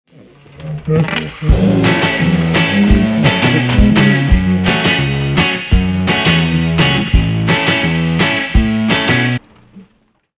Funny race start MTB crash
No riders were seriously hurt in any of these clips. Just a bruised egos.